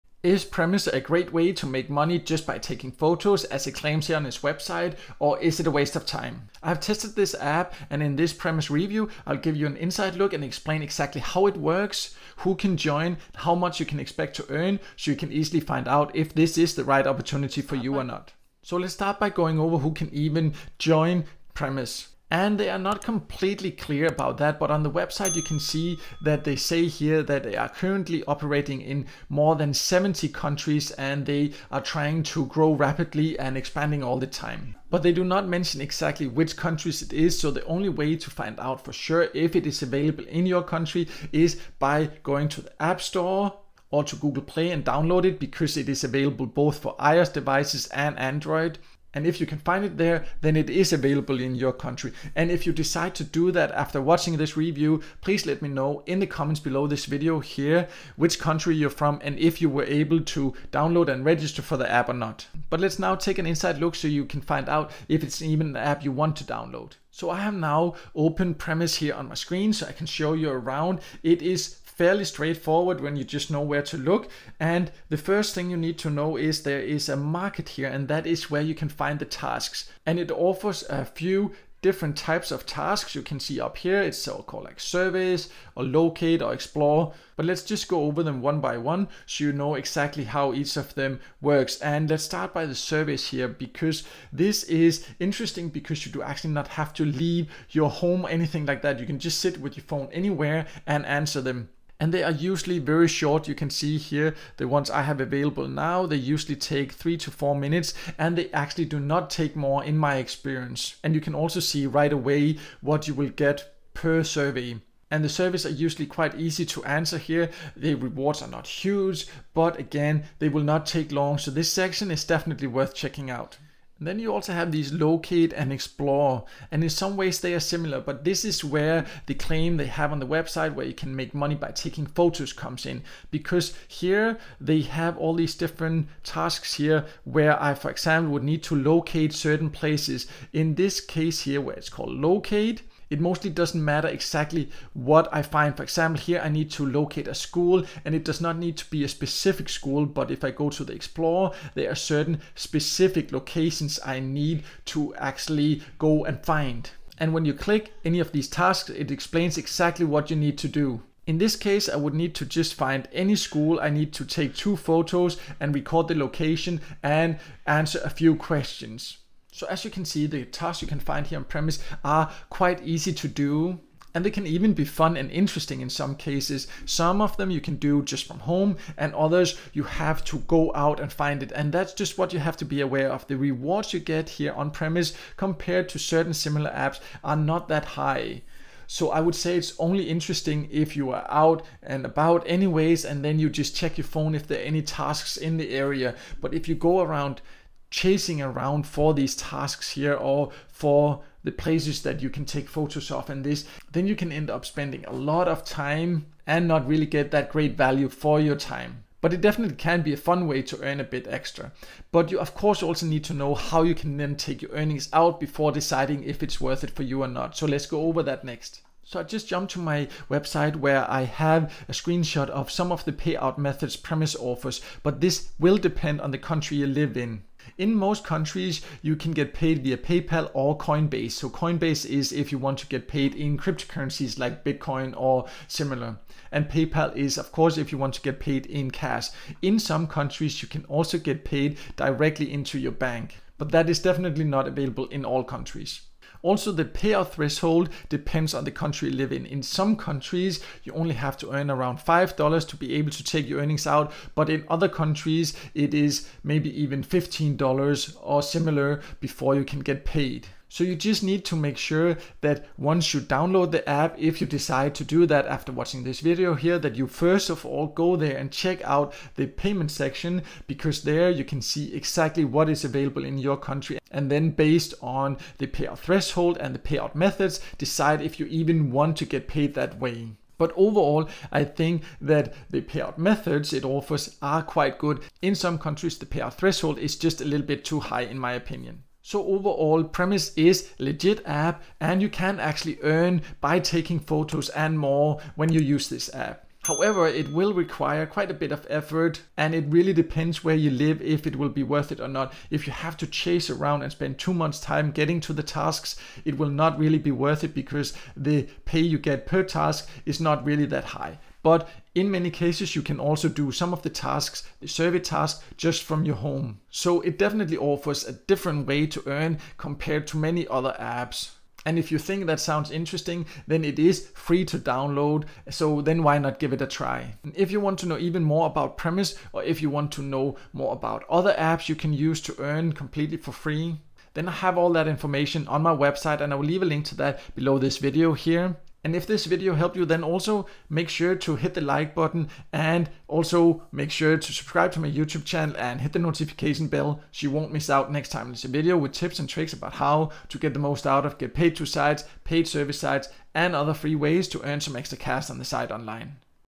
Premise App Review – Earn per Photo? (Yes, BUT…..)
This Premise App review will give you an inside look and show how you can use this app to earn by taking simple photos in your local area. Full details

Premise is an app that offers different ways to earn. You can earn by taking short surveys but the main earning method is by doing tasks in your local area that includes taking photos.

It is completely free to join and use and let me just reveal right away that it is legit as you can earn by using it.

However, you need to do it with the right expectations.

In this Premise App review and tutorial, I will give you an inside look and show all the details so you know exactly how much you can earn and what to expect.

One thing you need to consider is that the tasks you can do where you will get paid to take photos will take a bit of time. The tasks themselves are easy and fast to do, but you need to go to certain locations to take the photos.

Therefore, you need to choose your tasks wisely so you do not spend too much time chasing around for the locations as you will then not get good value for your time as the rewards are fairly small.

But if you live in an area with plenty of tasks and are out and about anyway, then Premise App can be a fun and different way to earn a bit extra.

However, if you are looking for more effective apps to earn and some where you can earn without having to go to certain locations, then there are better options. I will leave links to lists of some good options below:

0:00 Intro to Premise App
0:18 Who can join
1:06 How to earn with Premise App
2:03 How to earn by taking photos
2:49 How much can you earn
3:38 Premise App payment methods
4:49 Final verdict

Get the Ultimate Guide to Make Money with Paid Surveys and GPT Sites – it is 100% FREE: